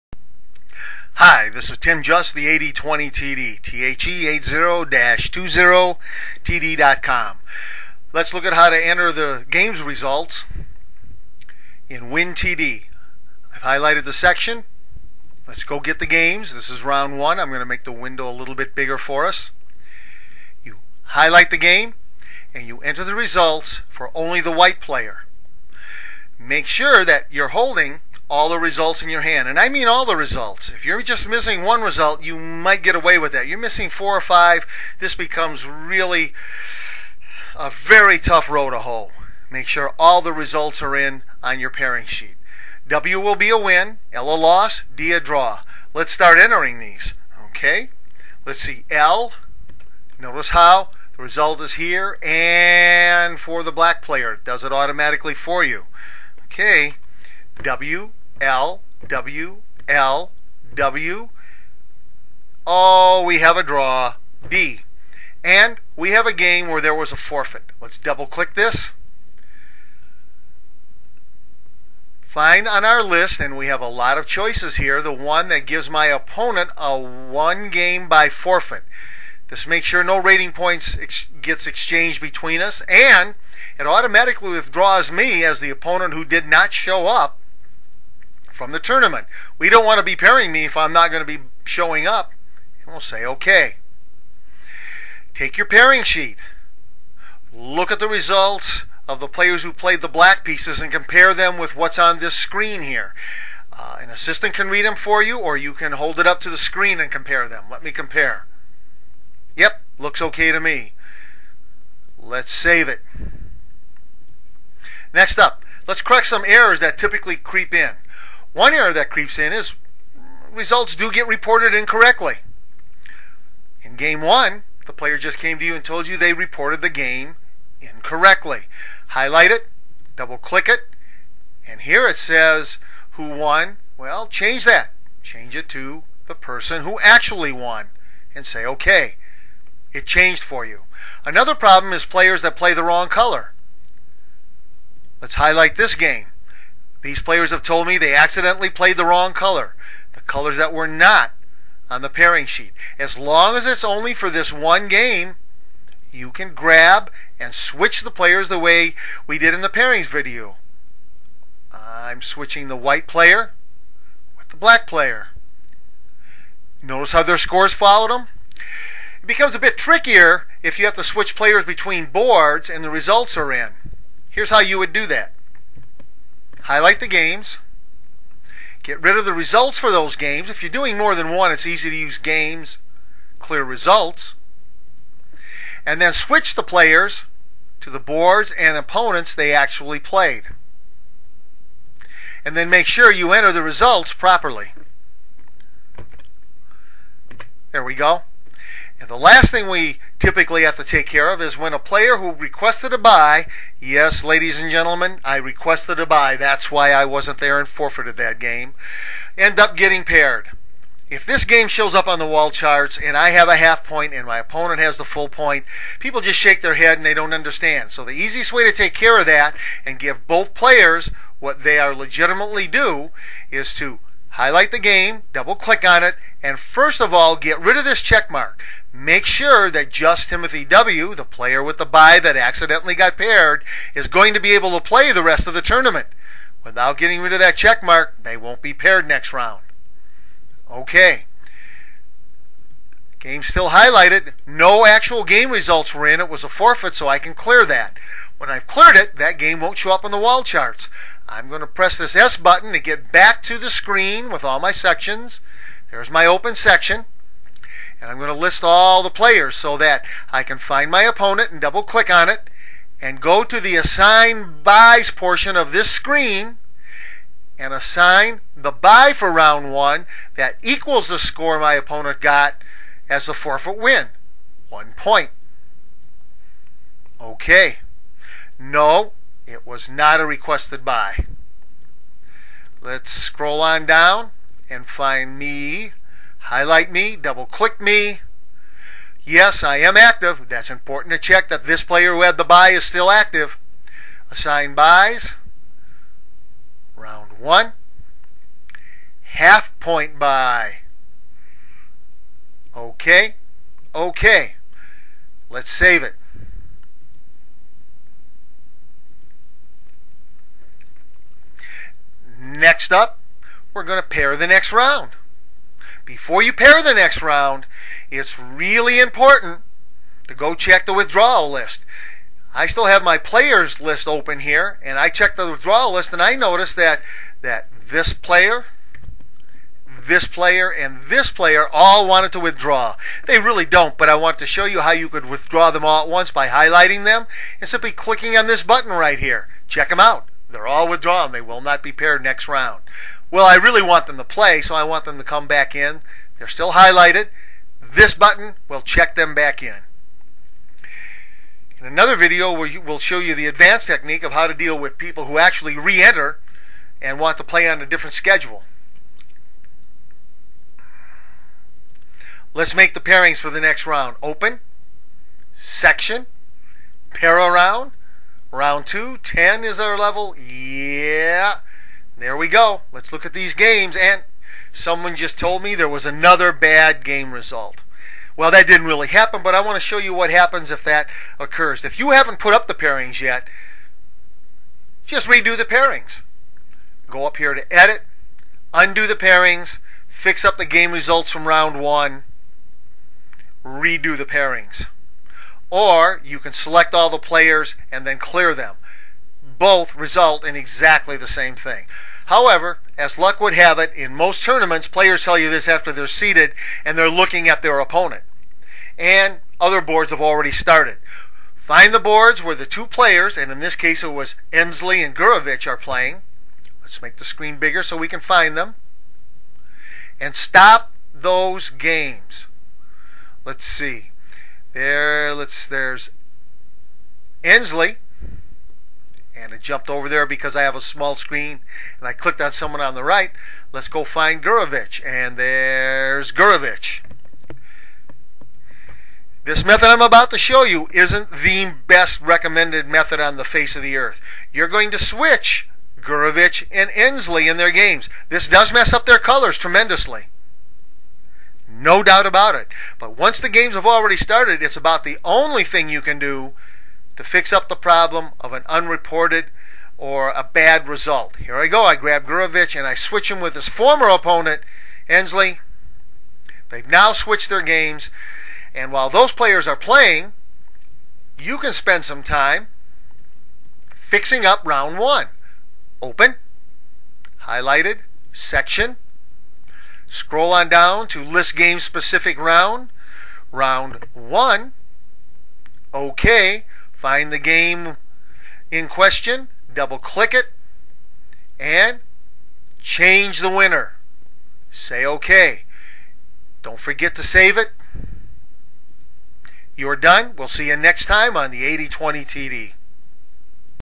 WinTD Basics: Game Results-Entering Them, Correcting Them
This is an improved sound version of an earlier video. This video teaches the user to enter individual chess game results for a round of a tournament using WinTD. The user will also learn how to deal with incorrect game results, mistaken pairings, and withdrawals.